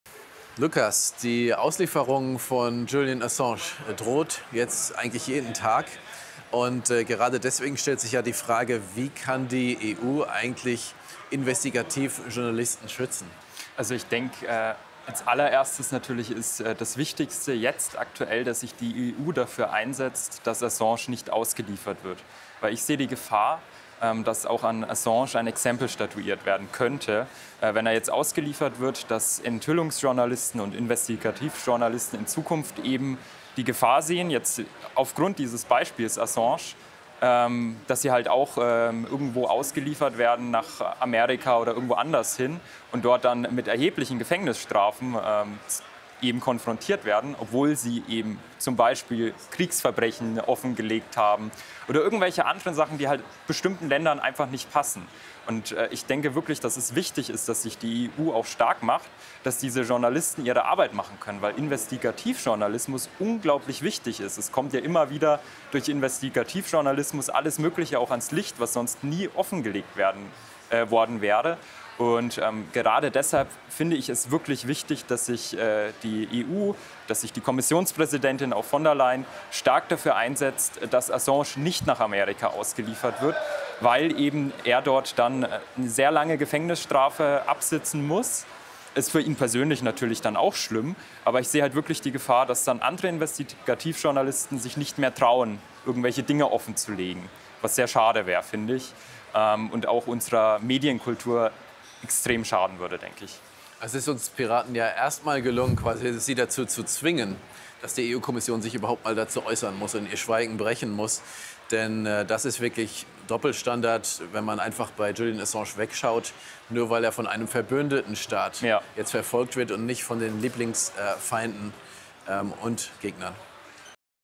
Free Assange: Warum Julian Assange freikommen muss
KurzGesagt: Warum Julian Assange freikommen muss - und warum wir Piraten das Schweigen der EU-Kommission gebrochen haben.